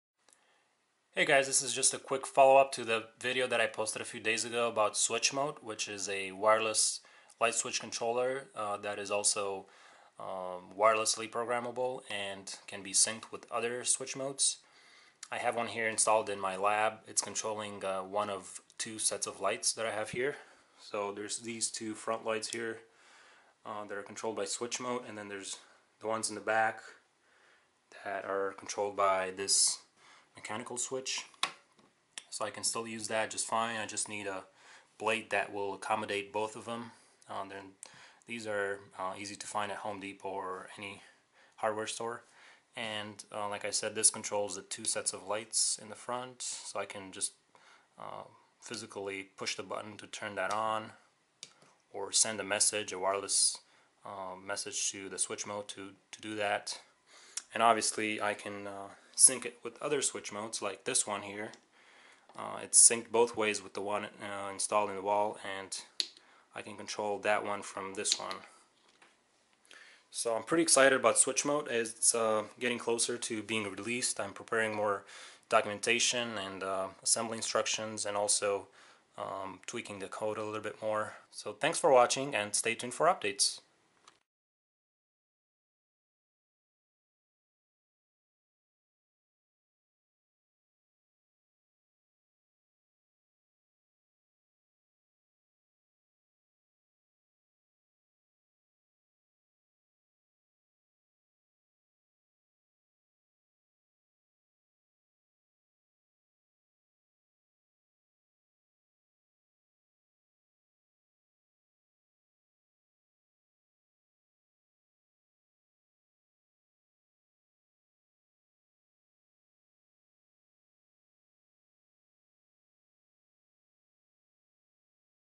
Installed SwitchMote demonstration
SwitchMote is a wireless light switch controller that can replace traditional mechanical light switches. It can be used to control lights physically as before or wirelessly, or even synchronize with other remote SwitchMotes to create home automation lighting scenes. The Moteino wireless Arduino clone enables it to be reprogrammed over the air.